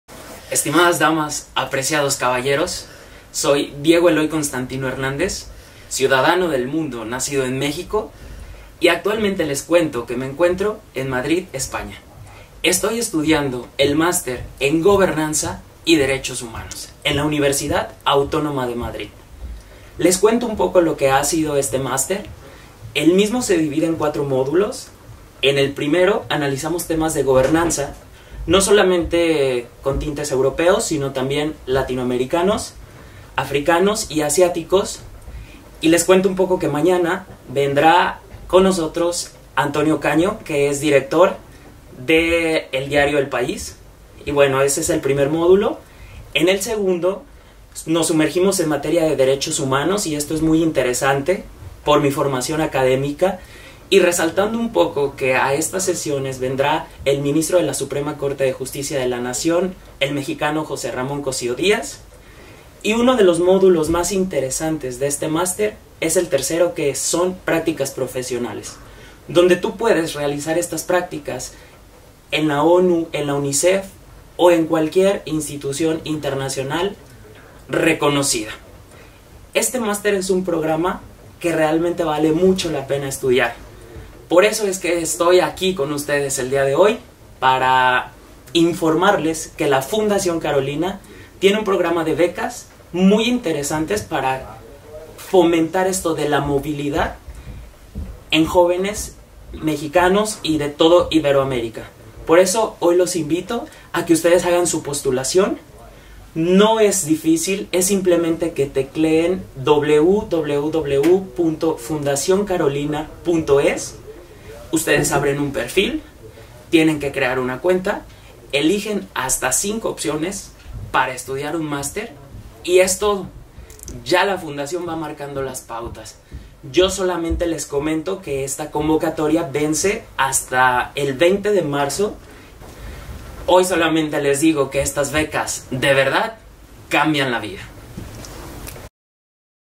Diego Eloy Constantino Hernández
Diego cuenta su experiencia como becario de Fundación Carolina en el Master en Gobernanza y Derechos Humanos de la Universidad Autónoma de Madrid